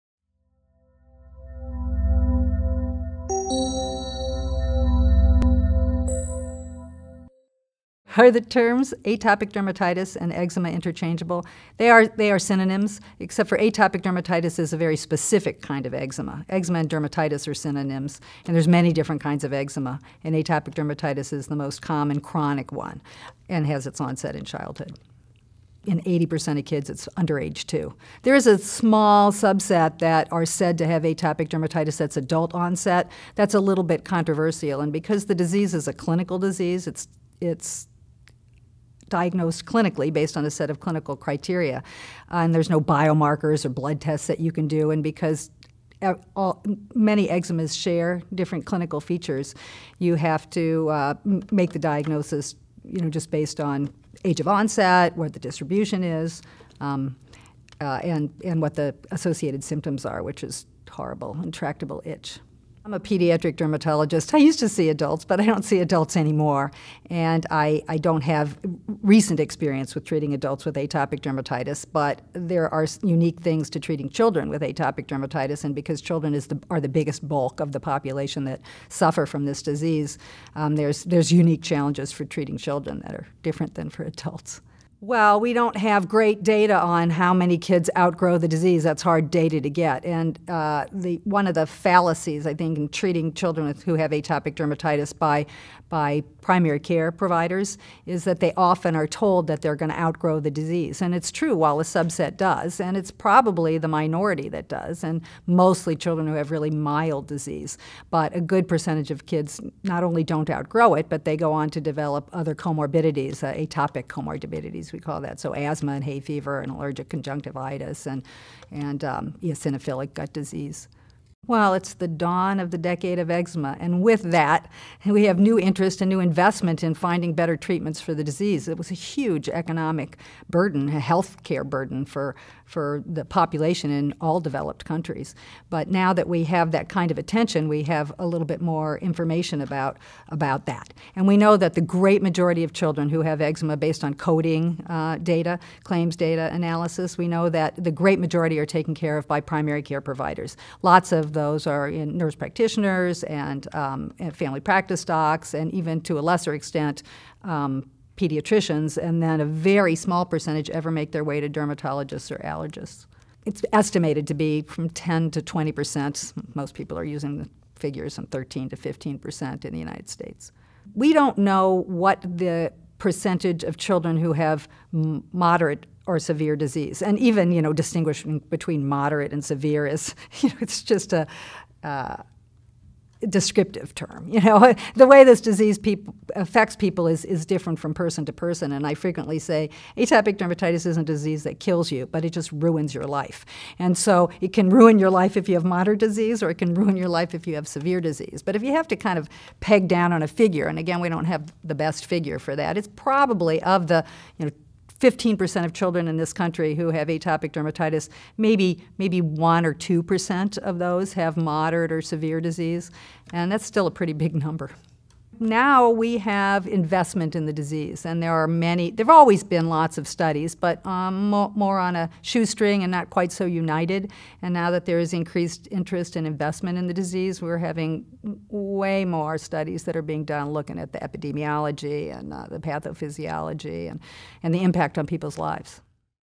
An Overview of Atopic Dermatitis
An overview of the treatment landscape of atopic dermatitis, with a comparison of the management of children to adults.